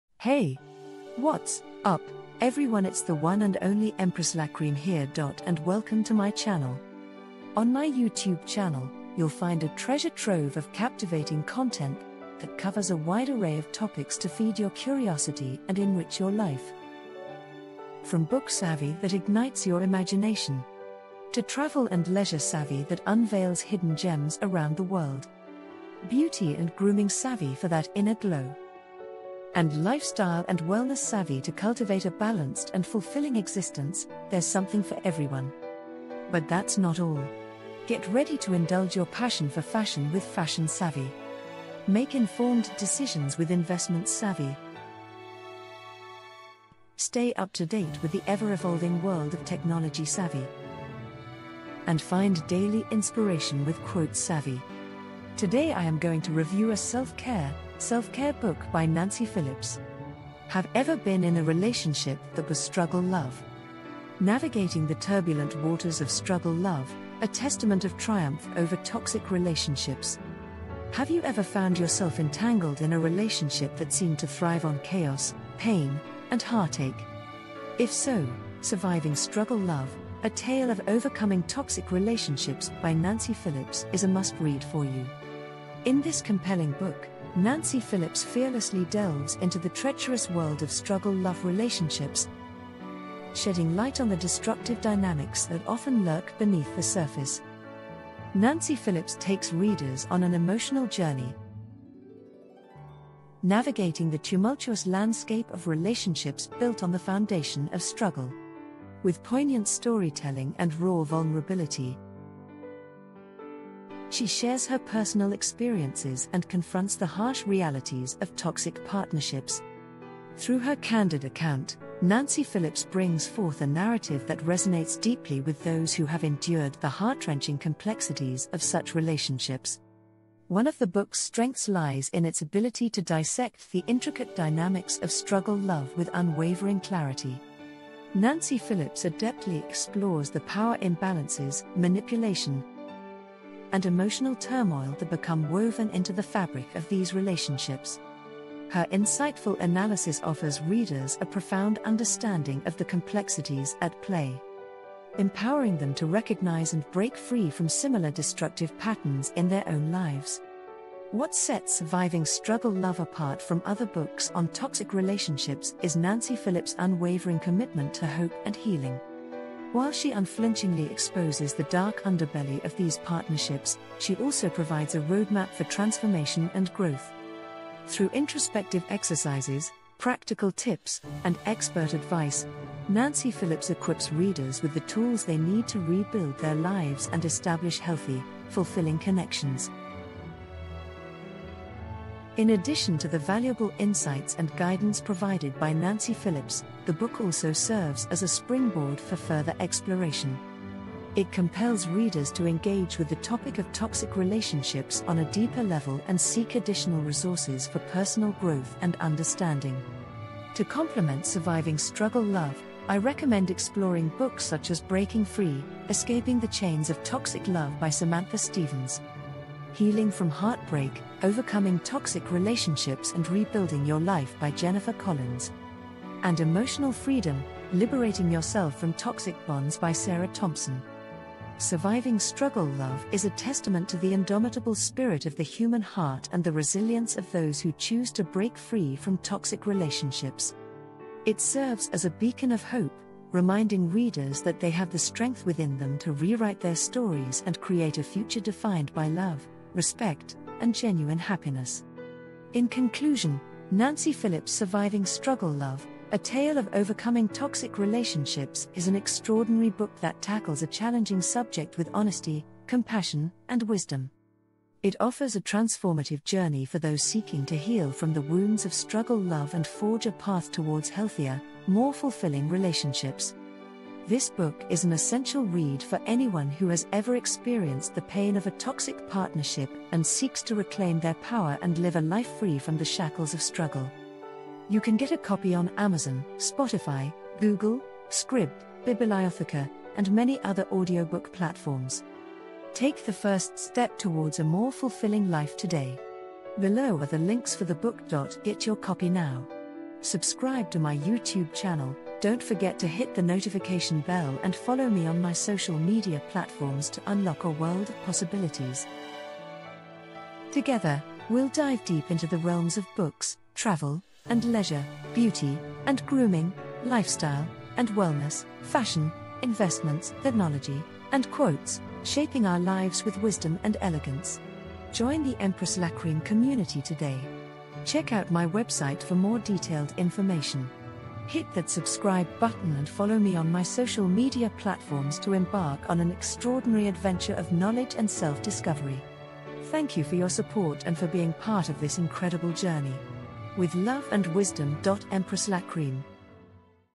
How To Survive 'struggle Love': A Tale Of Overcoming Toxic Relationships
Navigating the Turbulent Waters of "Struggle Love":
 A Testament of Triumph over Toxic Relationships
Have you ever found yourself entangled in a relationship that seemed to thrive on chaos, pain, and heartache? If so, "Surviving 'Struggle Love': A Tale of Overcoming Toxic Relationships" by Nancy Phillips is a must-read for you. In this compelling book, Phillips fearlessly delves into the treacherous world of "struggle love" relationships, shedding light on the destructive dynamics that often lurk beneath the surface.
Phillips takes readers on an emotional journey, navigating the tumultuous landscape of relationships built on the foundation of struggle. With poignant storytelling and raw vulnerability, she shares her personal experiences and confronts the harsh realities of toxic partnerships. Through her candid account, Phillips brings forth a narrative that resonates deeply with those who have endured the heart-wrenching complexities of such relationships.
One of the book's strengths lies in its ability to dissect the intricate dynamics of "struggle love" with unwavering clarity. Phillips adeptly explores the power imbalances, manipulation, and emotional turmoil that become woven into the fabric of these relationships. Her insightful analysis offers readers a profound understanding of the complexities at play, empowering them to recognize and break free from similar destructive patterns in their own lives.
What sets "Surviving 'Struggle Love'" apart from other books on toxic relationships is Phillips' unwavering commitment to hope and healing. While she unflinchingly exposes the dark underbelly of these partnerships, she also provides a roadmap for transformation and growth. Through introspective exercises, practical tips, and expert advice, Phillips equips readers with the tools they need to rebuild their lives and establish healthy, fulfilling connections.
In addition to the valuable insights and guidance provided by Phillips, the book also serves as a springboard for further exploration. It compels readers to engage with the topic of toxic relationships on a deeper level and seek additional resources for personal growth and understanding.